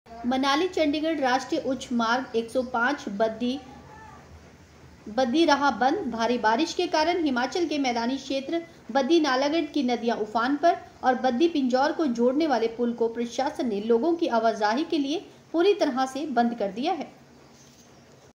बद्दी पिंजौर को जोड़ने वाले पुल को प्रशासन ने लोगो की आवाजाही के लिए पूरी तरह बंद के दिया
मनाली चंडीगढ़ राष्ट्रीय उच्च मार्ग 105 बद्दी बंद भारी बारिश के कारण  हिमाचल के मैदानी क्षेत्र   बद्दी नालागढ़ की नदियां उफान पर और बद्दी पिंजौर को जोड़ने वाले पुल को प्रशासन ने लोगो की आवाजाही के लिए पूरी तरह बंद के दिया